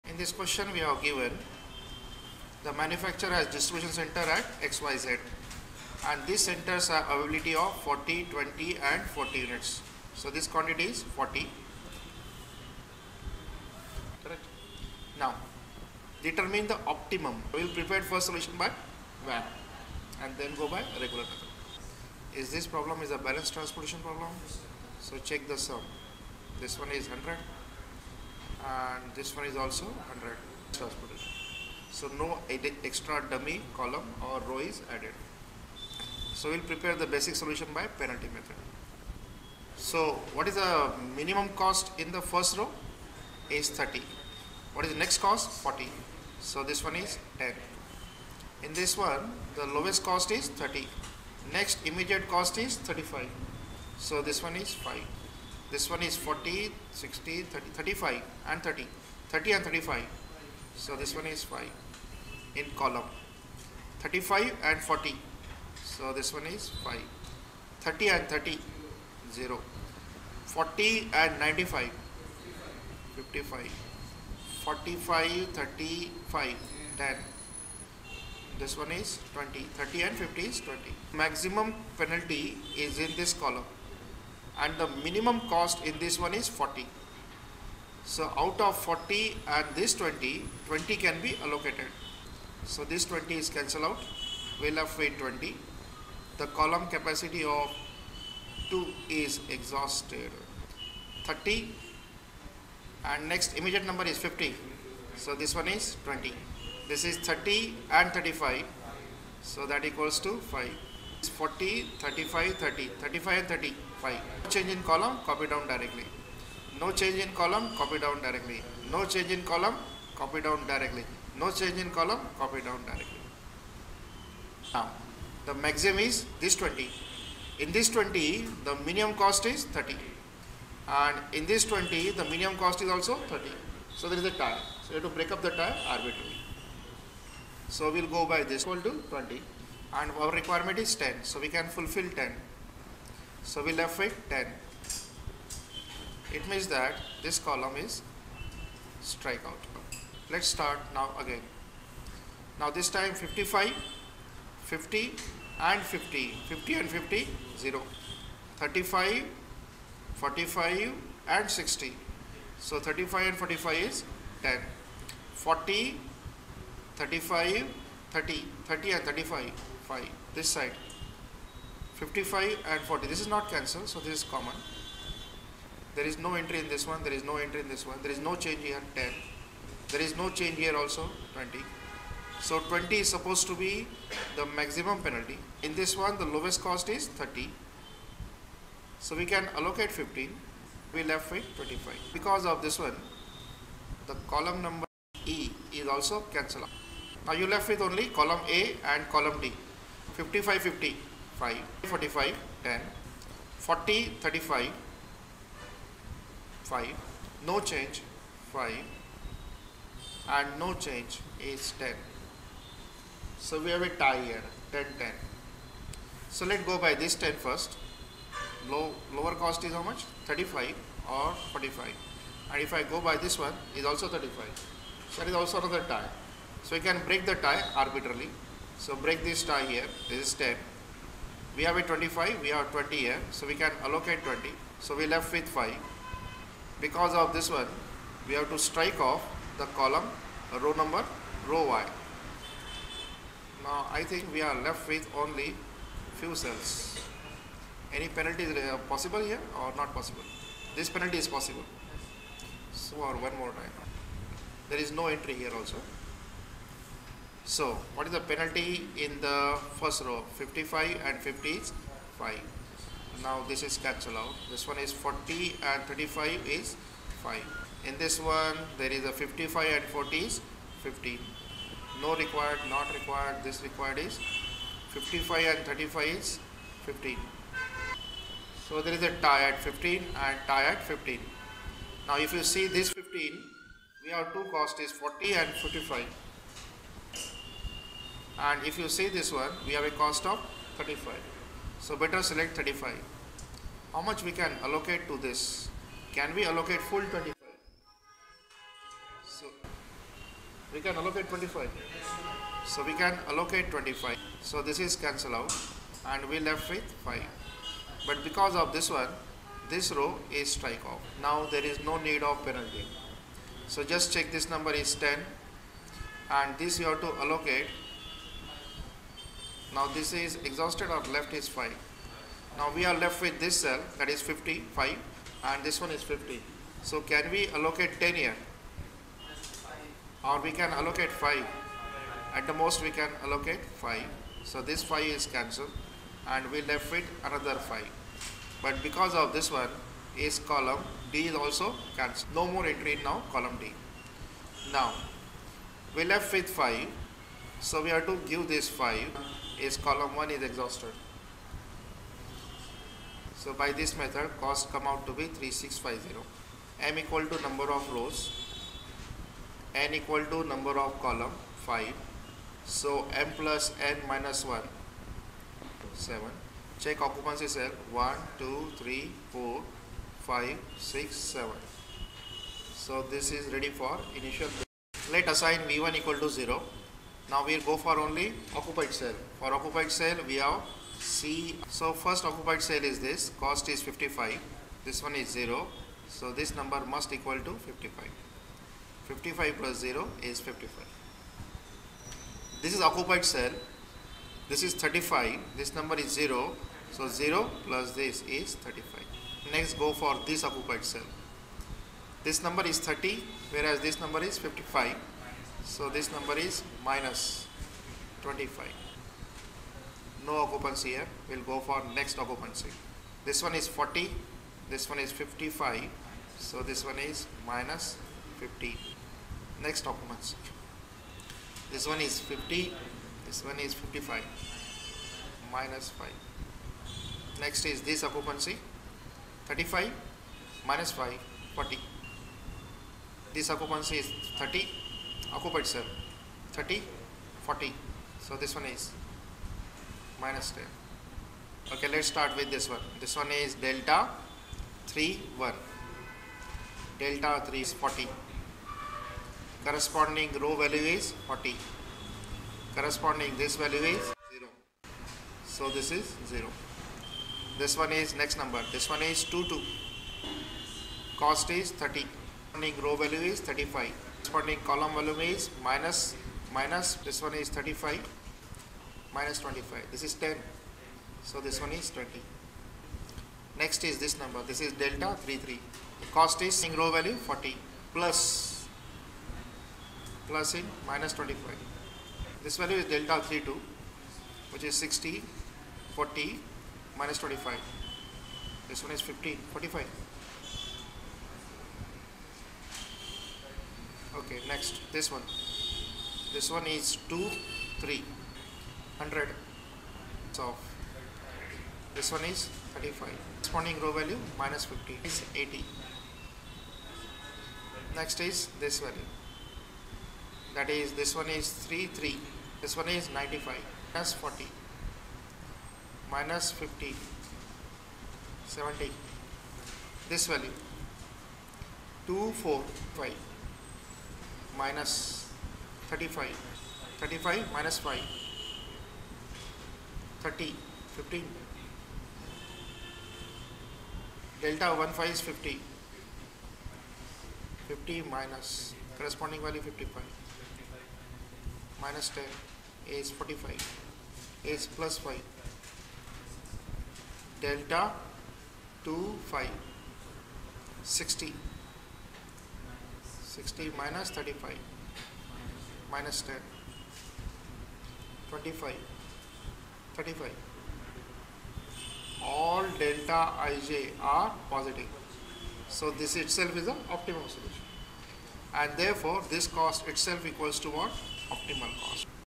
05 Trans Model |Optimal solution by MODI Method| u-v method |Modified Distribution Method
The objective of the transportation problem is to transport various quantities of a single homogenous commodity, which are initially stored at various origins to various destinations in such a way that the total transportation cost is minimum.
Basic Feasible solution: A feasible solution to a m-origin, n-destination problem is said to be basic if the number of positive allocations are equal to (m+n-1).
 Feasible Solution: A set of positive individual allocations which simultaneously removes deficiencies is called a feasible solution.
Optimal Solution: A feasible solution (not basically basic) is said to be optimal if it minimizes the total transportation cost.
Methods of solving Transportation Problems
1. North- West Corner Rule method
2. Row-minima Method
 3. Column minima method
4. Matrix Minima Method or least cost method
 5. Vogel's Approximation method (VAM)
There are two methods of giving optimality test:
(a) Stepping Stone Method.
(b) Modified Distribution Method, generally known as MODI method. Transportation Model Optimal sol by MODI Method Some Numerical are solved to help out clear understanding of MODI method